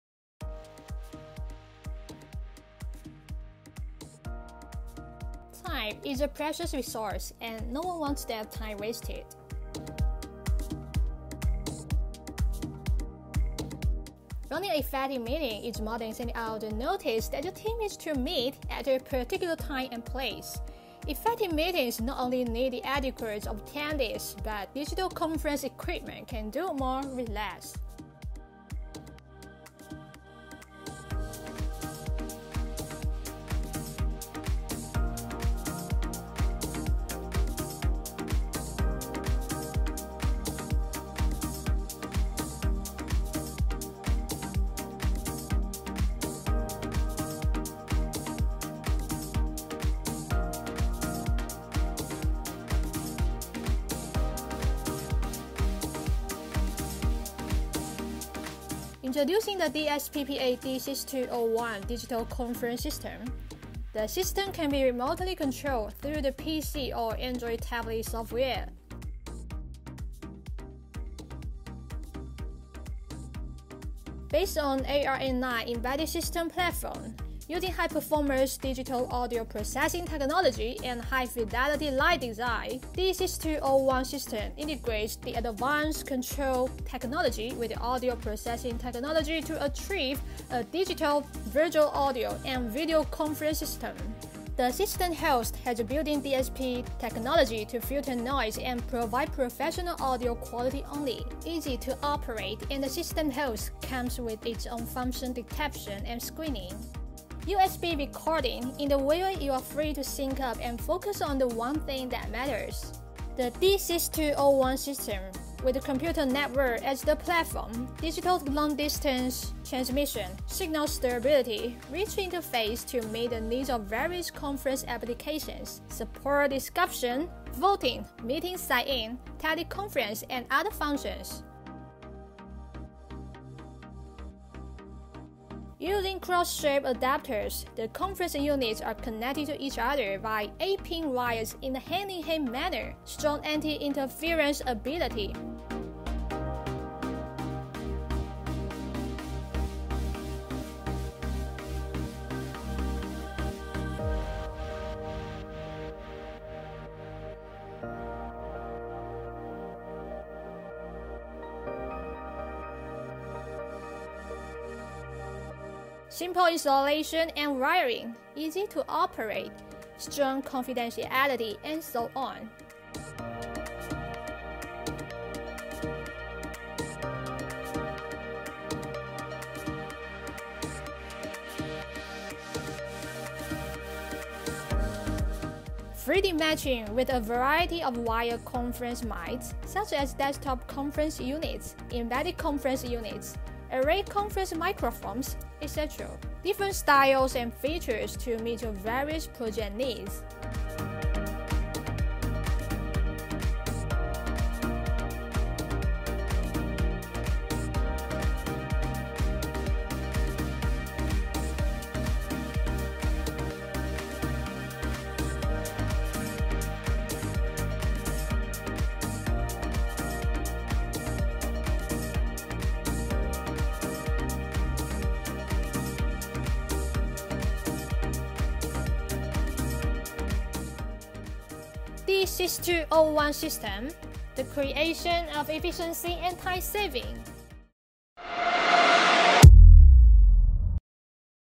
DSPPA | D6201 Intelligent Conference System Introduction
Interested in the D6201 Audio Conference System?

Time is a precious resource, and no one wants their time wasted.
Running an effective meeting is more than sending out a notice that your team is to meet at a particular time and place. Effective meetings not only need the etiquette of attendees, but digital conference equipment can do more with less. And now DSPPA offers audio solutions for conference rooms of all sizes.